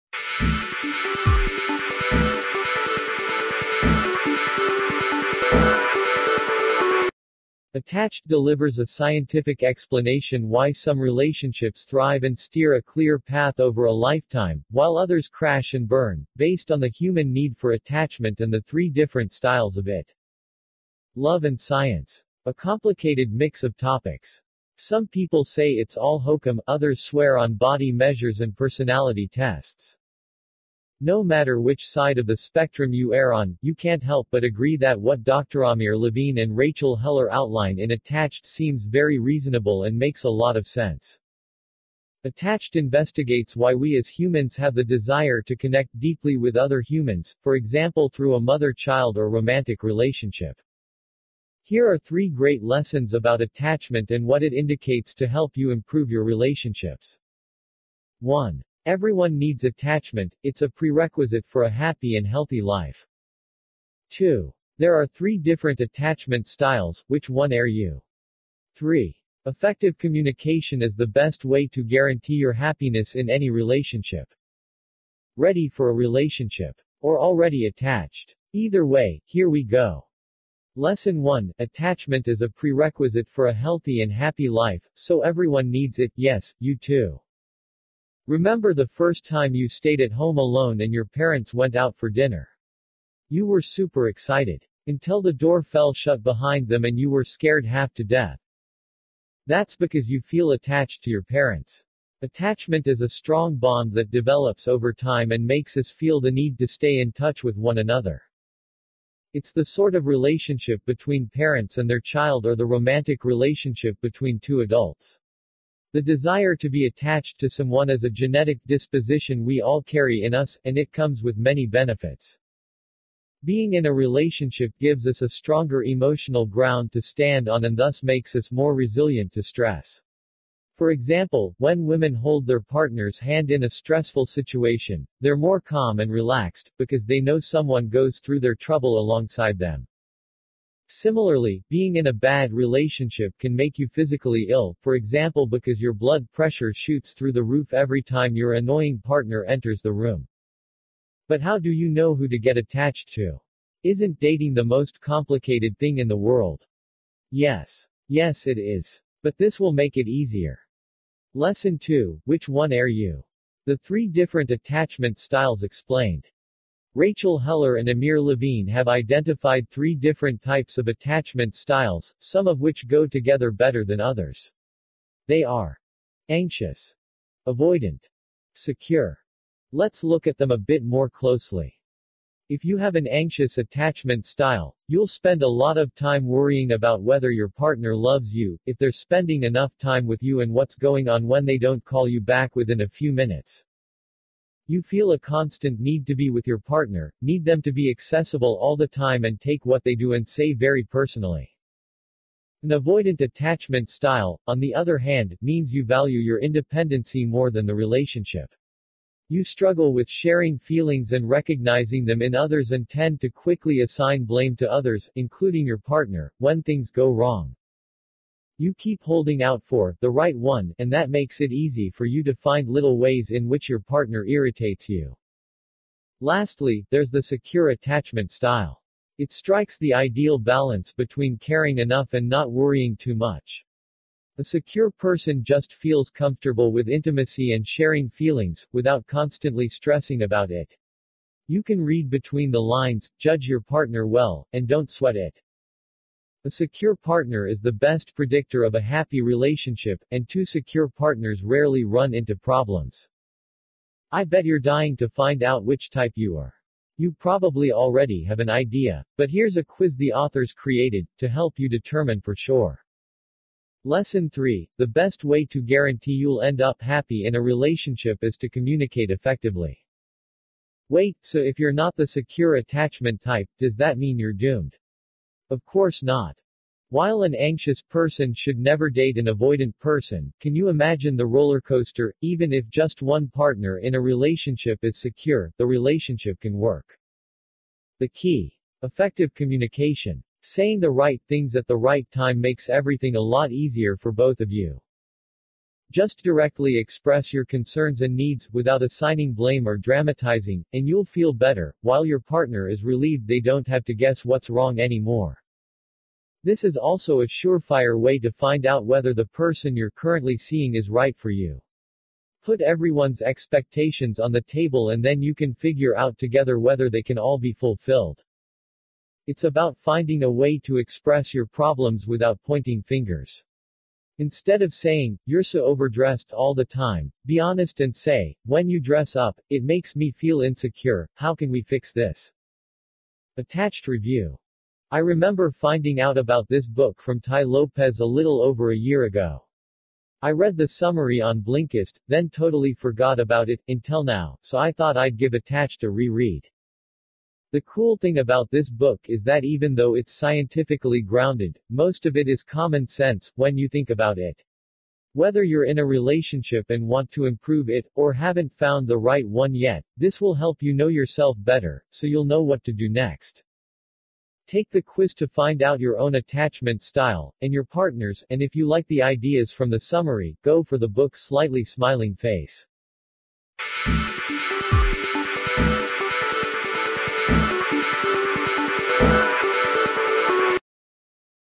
Attached Summary
Attached delivers a scientific explanation why some relationships thrive and steer a clear path over a lifetime, while others crash and burn, based on the human need for attachment and the three different styles of it.
Love and science. A complicated mix of topics. Some people say it’s all hokum, others swear on body measures and personality tests.

No matter which side of the spectrum you err on, you can’t help but agree that what Dr. Amir Levine and Rachel Heller outline in Attached seems very reasonable and makes a lot of sense.

Attached investigates why we as humans have the desire to connect deeply with other humans, for example through a mother-child or romantic relationship.

Here are 3 great lessons about attachment and what it indicates to help you improve your relationships:

    1.Everyone needs attachment, it’s a prerequisite for a happy and healthy life.
    2.There are 3 different attachment styles, which one are you?
    3.Effective communication is the best way to guarantee your happiness in any relationship.

Ready for a relationship? Or already attached? Either way, here we go!